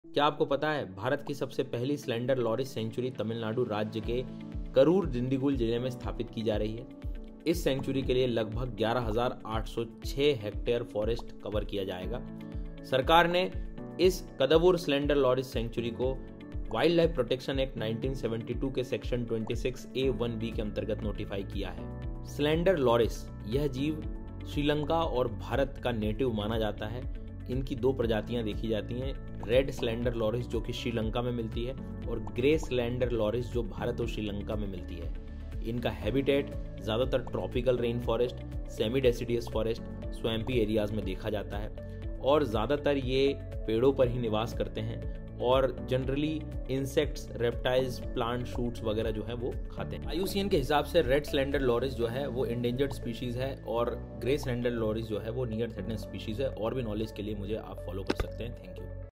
India’s first Slender Loris Sanctuary!
India’s first Slender Loris Sanctuary to be established in Tamil Nadu

• India's first-ever sanctuary for the endangered Slender Loris will be set up in the Karur & Dindigul districts of Tamil Nadu.
• The sanctuary is to cover Vedasandur, Dindigul East, and Natham taluks in the Dindigul district and Kadavur taluk in the Karur district.